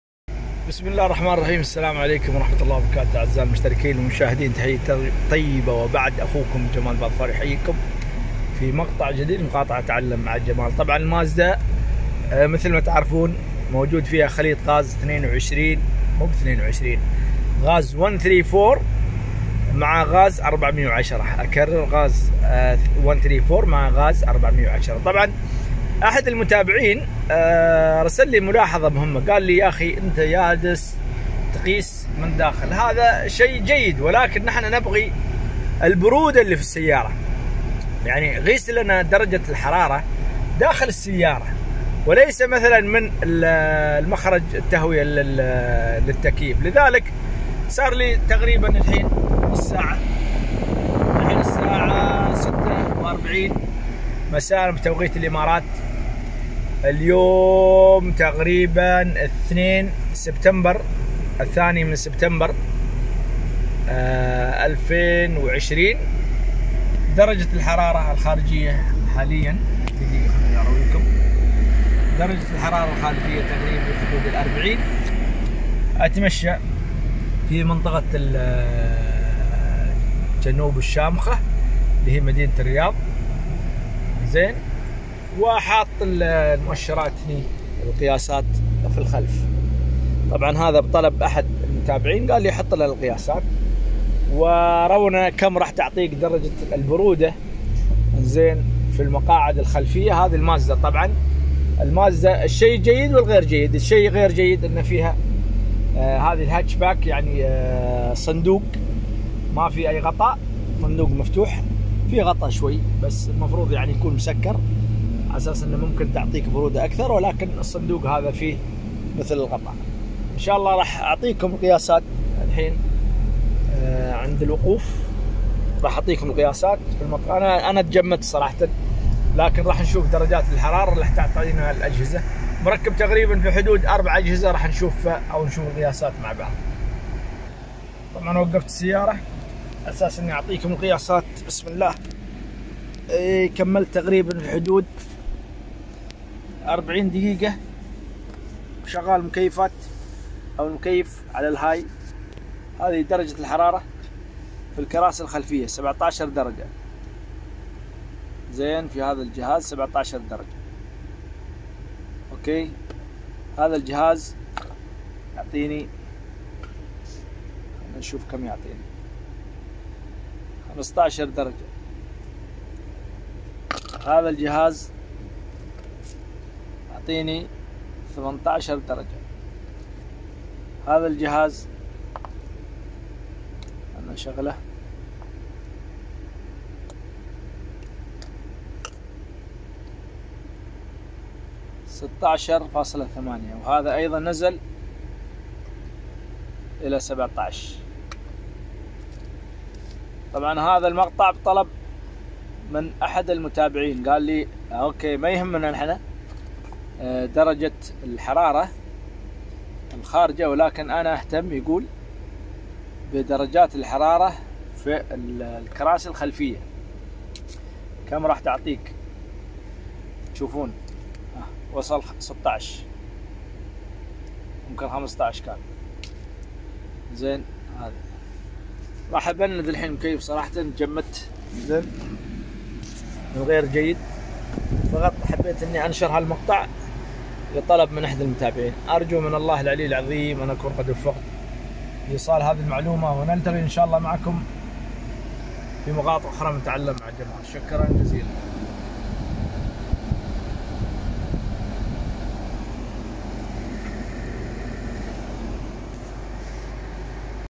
المازدا 💪 ودرجات الحرارة داخل المقصورة 🥶🥶🥶
نظام التبريد في المازدا حالياً يعمل بخليط من غاز 134 + 410